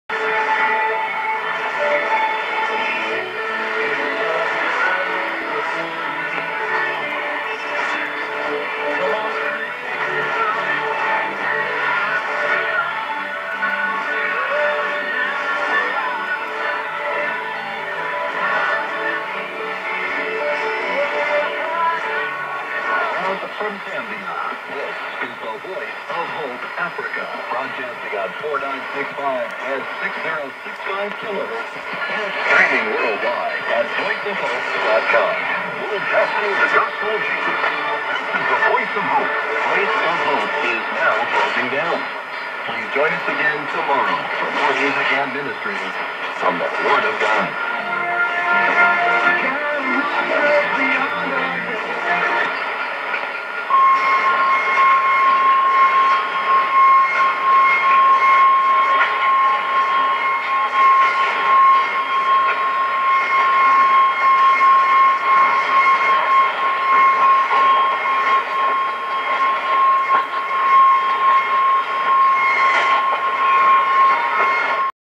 The Voice of Hope Africa, 6065 kHz , 2017July25 2157
2017July25 21:57 UTC 6065 kHz
The Voice of Hope Africa
good signal S9 R3  severe Fading-2 and noise.
Via the 100kW Transmitter from Makeni Ranch / Zambia in english Language.
Rec. in Germany JN49AC  Loop  R1000  QRB 7400km